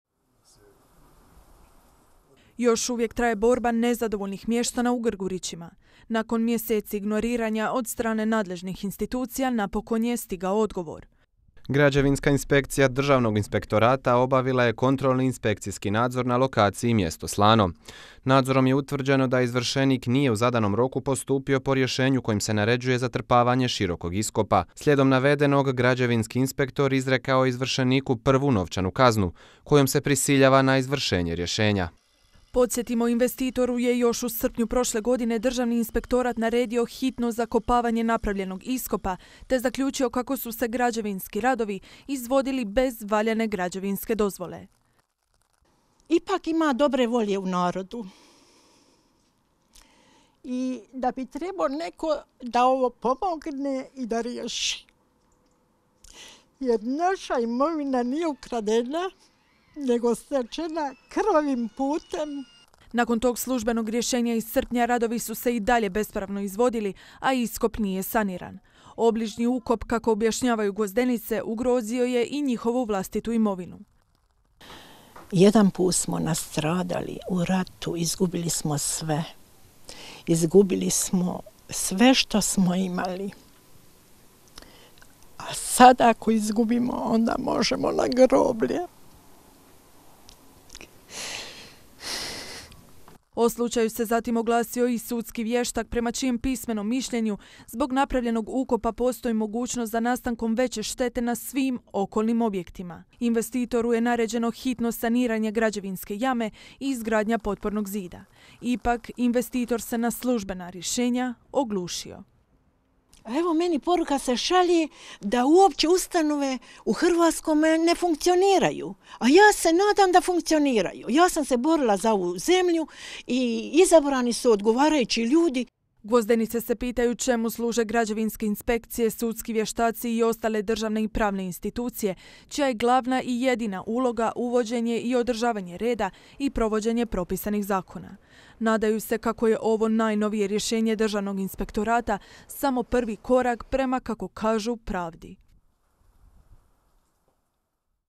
INVESTITORU STIGLO DRUGO RJEŠENJE I KAZNA, A I DALJE SE OGLUŠUJE!
I dalje pratimo situaciju u mjestu Grgurići pokraj Slanog. Nakon više mjeseci šutnje, o slučaju se ponovno oglasio Državni inspektorat. Gvozdenice se nadaju da je ovo prvi korak prema rješavanju njihovog problema.